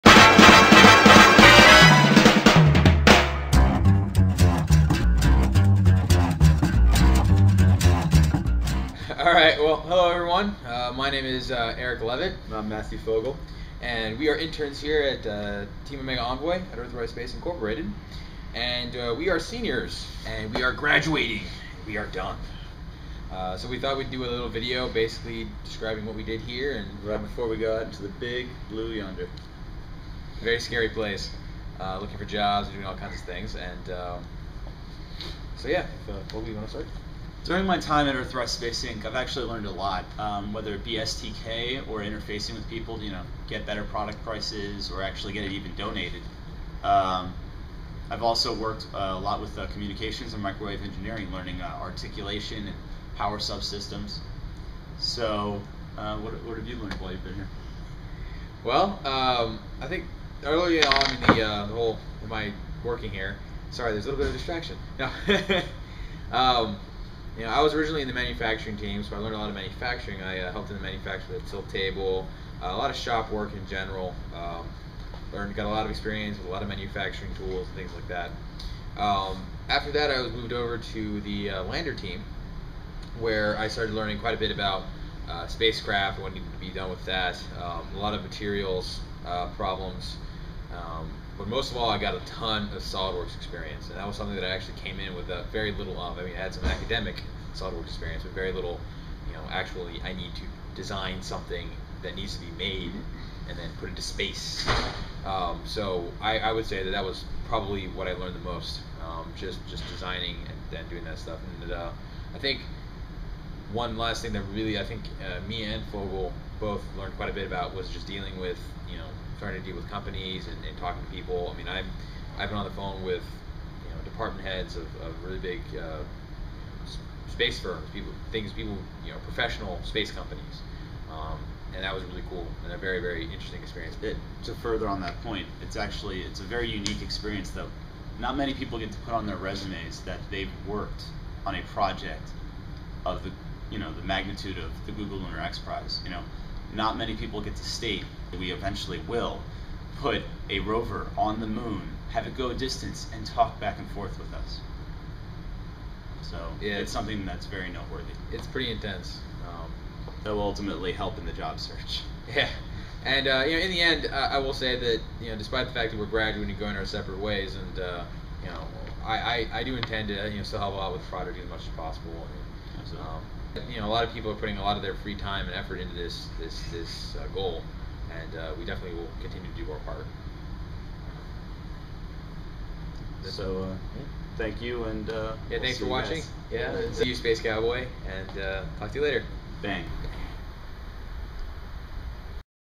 Wild Blue Yonder
Matthew Fogel and Eric Leavitt bids farewell to everyone and describes their learning experience during the 7 months they worked with Earthrise Space, Inc.